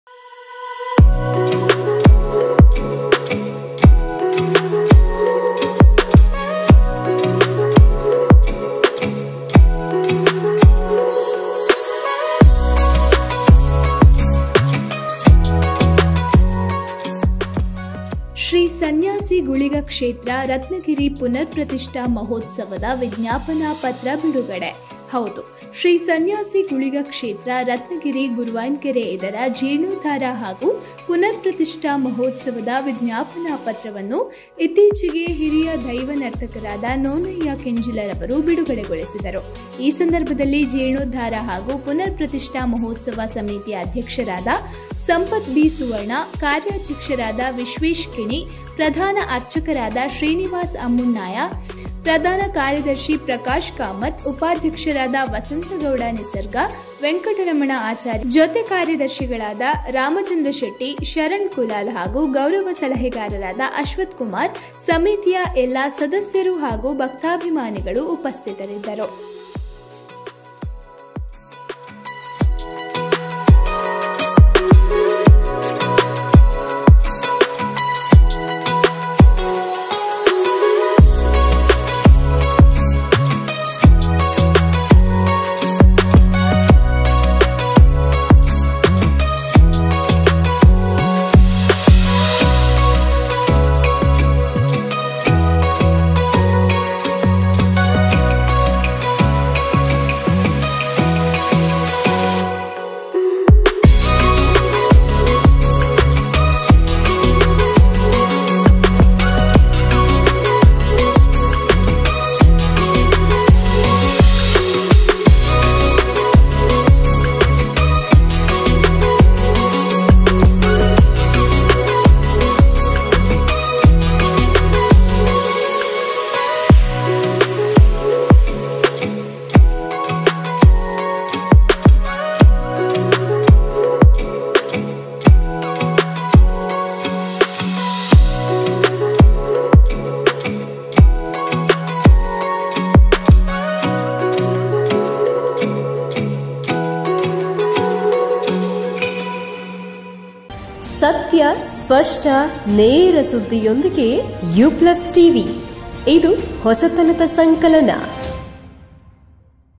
ಶ್ರೀ ಸನ್ಯಾಸಿ ಗುಳಿಗ ಕ್ಷೇತ್ರ, ರತ್ನಗಿರಿ ಪುನರ್ ಪ್ರತಿಷ್ಠಾ ಮಹೋತ್ಸವದ ವಿಜ್ಞಾಪನಾ ಪತ್ರ ಬಿಡುಗಡೆ||U PLUS TV
ಶ್ರೀ ಸನ್ಯಾಸಿ ಗುಳಿಗ ಕ್ಷೇತ್ರ, ರತ್ನಗಿರಿ ಪುನರ್ ಪ್ರತಿಷ್ಠಾ ಮಹೋತ್ಸವದ ವಿಜ್ಞಾಪನಾ ಪತ್ರ ಬಿಡುಗಡೆ

ಹಿರಿಯ ದೈವನರ್ತಕರಾದ ಶ್ರೀ ನೋಣಯ್ಯ ಕೆಂಜಿಲ ಅವರಿಂದ ಬಿಡುಗಡೆ

ಸನ್ಯಾಸಿ ಗುಳಿಗ ಕ್ಷೇತ್ರ, ರತ್ನಗಿರಿ ,ಗುರುವಾಯನಕೆರೆಯ  ಜೀರ್ಣೋದ್ಧಾರ ಹಾಗೂ ಪುನರ್ ಪ್ರತಿಷ್ಠಾ ಮಹೋತ್ಸವ